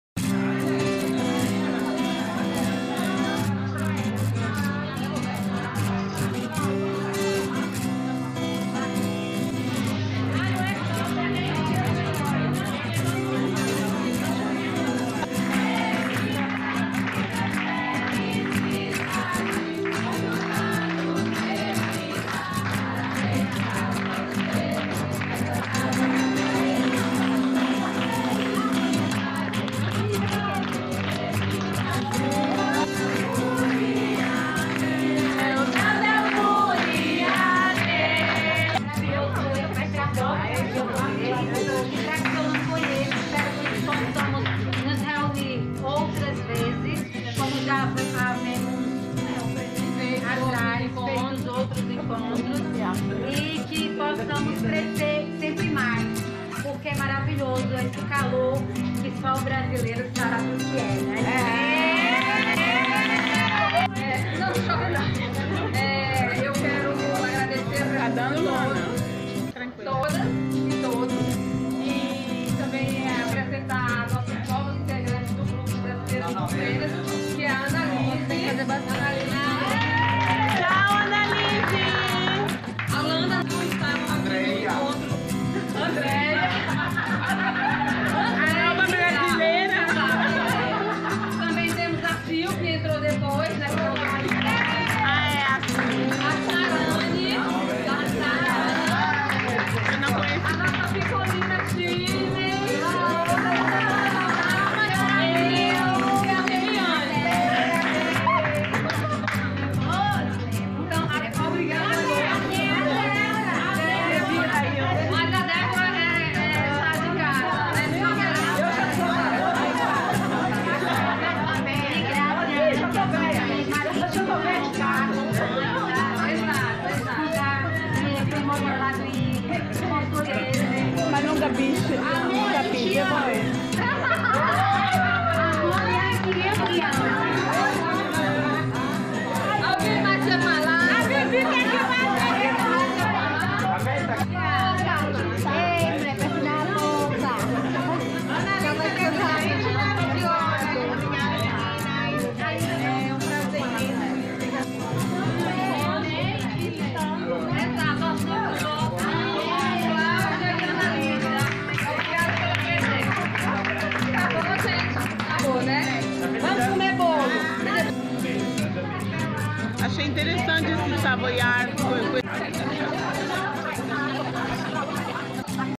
ANIVERSÁRIANTE DO MÊS DO GRUPO MULHERES Brasileiras na regiāo Veneto na Itália
NO NOSSO ENCONTRINHO DE MULHERES BRASILEIRAS QUE VIVEM NA REGIĀO VENETO - ITÁLIA
ESTE MÊS FORAM A BELL, VIVIANE E LUCIENE.
FOI TUDO MUITO LINDO E A TORTA FOI FEITA PELA CRISTIANE.UMA DELICIA

ESPERO QUE GOSTEI

GRUPO FACEBOOK DE MULHERES BRASILEIRAS QUE VIVEM NA ITÁLIA
ESTE VIDEO É DA REGIAO VENETO

DEIXEM NO COMENTÁRIO A SUA OPINIĀO

BEIJOS